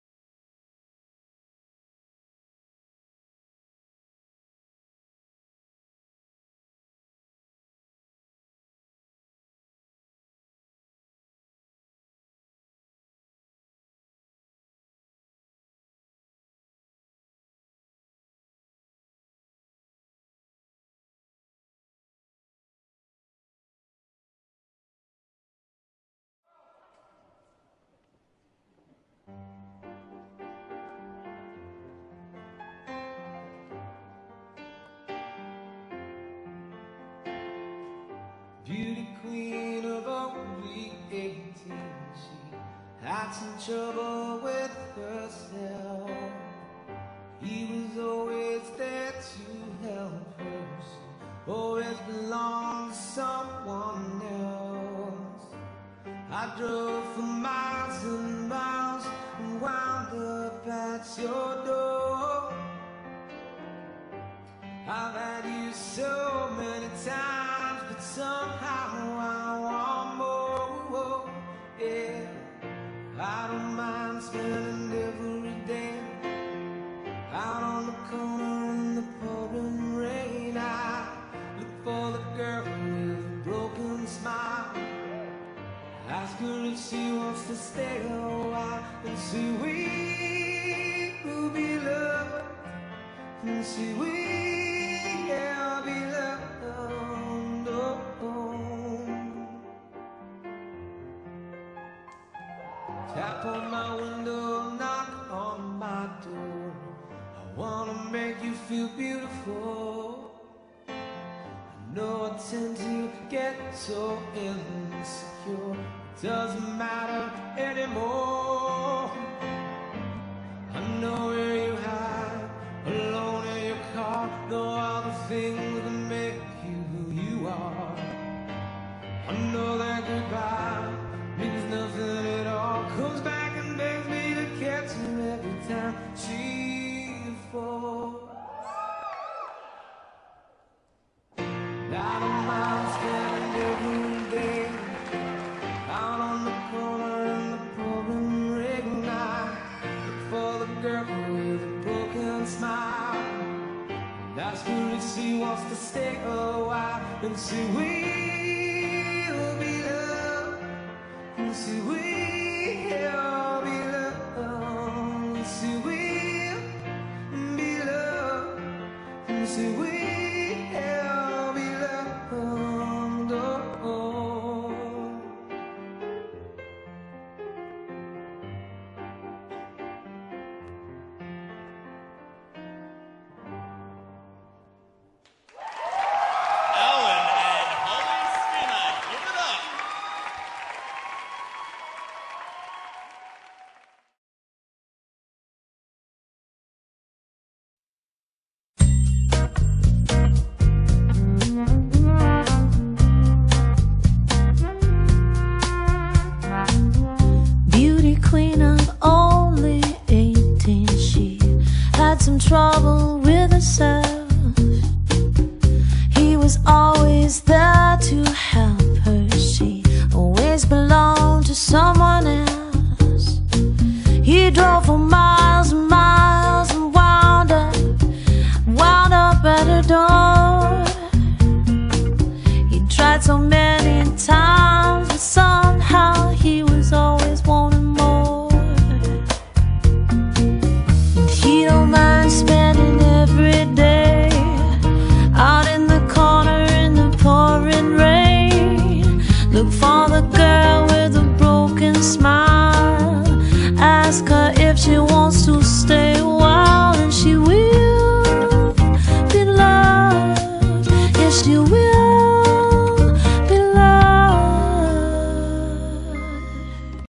Audacity Dance Canberra workshops and Dinner Dance special guests
Allan and Holly Skinner performed their showcase at the Audacity Dance Canberra weekender, 25/2/12.    For the best in WCS dance lessons - go